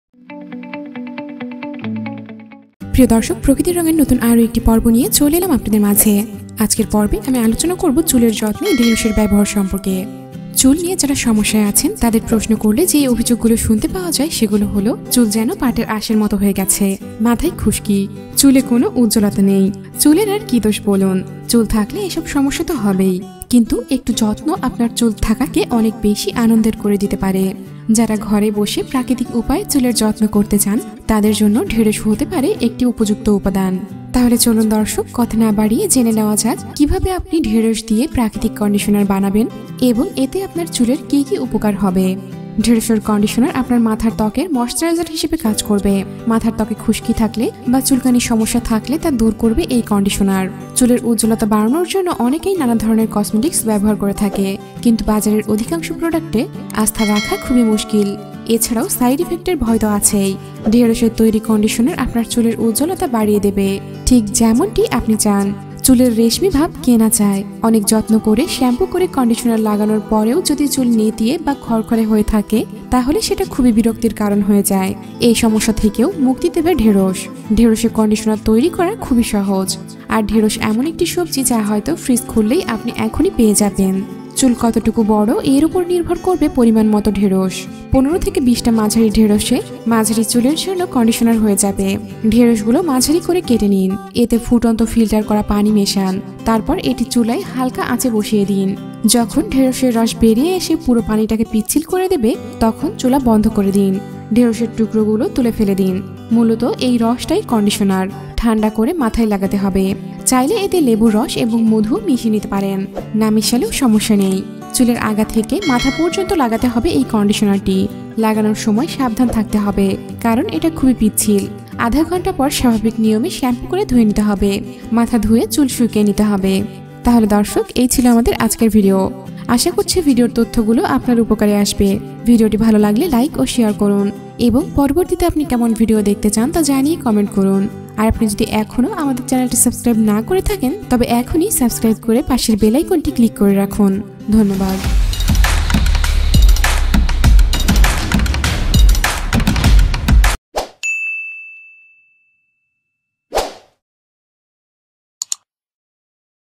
চুলের যত্নে ঢেঁড়স! অবাক হচ্ছেন? দেখলেই বুজবেন! রেশমি চুল পেতে ঢেঁড়সের কন্ডিশনার। প্রকৃতির রং
প্রকৃতির ব্যবহার এবং এর গুনাবলি সম্পর্কে আপনাদের জানাতে আমাদের এই ক্ষুদ্র প্রচেষ্টা মাত্র। সবস্ক্রাইব করে সাথে থাকুন। ধন্যবাদ

ঢেঁড়স দিয়ে কীভাবে প্রাকৃতিক কন্ডিশনার বানাবেন সেটা দেখার আগে দেখে নিন এতে আপনার চুলের কী কী উপকার হবে।

* ঢেঁড়সের কন্ডিশনার আপনার মাথার ত্বকের মশ্চেরাইজার হিসেবে কাজ করবে। মাথার ত্বকে খুশকি থাকলে বা চুলকানির সমস্যা থাকলে তা দূর করবে এই কন্ডিশনার।

* চুলের উজ্জ্বলতা বাড়ানোর জন্য অনেকেই নানা ধরনের কসমেটিক ব্যবহার করে থাকেন। কিন্তু বাজারের অধিকাংশ প্রডাক্টের প্রতি আস্থা রাখাও মুশকিল এখন। এছাড়া সাইড ইফেক্টের ভয় তো আছেই। ঢেঁড়সের তৈরি কন্ডিশনার আপনার চুলের উজ্জ্বলতা বাড়িয়ে দেবে, ঠিক যেমনটি আপনি চান।

* চুলের রেশমি ভাব কে না চান? অনেক যত্ন করে শ্যাম্পু করে কন্ডিশনার লাগানোর পরও চুল যদি নেতিয়ে থাকে বা খড়খড়ে হয়ে থাকে তাহলে সেটা খুব বিরক্তির কারণ হয়ে দাঁড়ায়। এই সমস্যা থেকেও মুক্তি দেবে ঢেঁড়স!

ঢেঁড়সের কন্ডিশনার তৈরি করা খুবই সহজ। আর ঢেঁড়স এমন একটা সবজি যা হয়তো ফ্রিজে খুঁজলে এখনই আপনি পেয়ে যাবেন। চুল কতটুকু বড় এর উপর নির্ভর করে পরিমাণ মতো ঢেঁড়স নিন। ১৫ থেকে ২০ টা মাঝারি ঢেঁড়সে মাঝারি চুলের কন্ডিশনার হয়ে যাবে।

ঢেঁড়সগুলো মাঝারি করে কেটে নিন। এতে ফুটানো ফিল্টার করা পানি মেশান। তারপর এটি চুলায় হালকা আঁচে বসিয়ে দিন। যখন ঢেঁড়সের রস বের হয়ে এসে পুরো পানিটাকে পিচ্ছিল করে দেবে তখন চুলা বন্ধ করে দিন। ঢেঁড়সের টুকরো গুলো তুলে ফেলে দিন। মূলত এর রসটাই কন্ডিশনার। ঠাণ্ডা করে এটা মাথায় লাগাতে হবে। চাইলে এতে লেবুর রস বা মধু মিশিয়ে নিতে পারেন। না মেশালেও সমস্যা নেই।

চুলের আগা থেকে মাথা পর্যন্ত লাগাতে হবে কন্ডিশনারটি। লাগানোর সময় সাবধান থাকতে হবে কারণ এটি খুবই পিচ্ছিল। আধা ঘণ্টা পর স্বাভাবিক নিয়মে শ্যাম্পু করে নিতে হবে। মাথা ধুয়ে চুল শুকিয়ে নিতে হবে।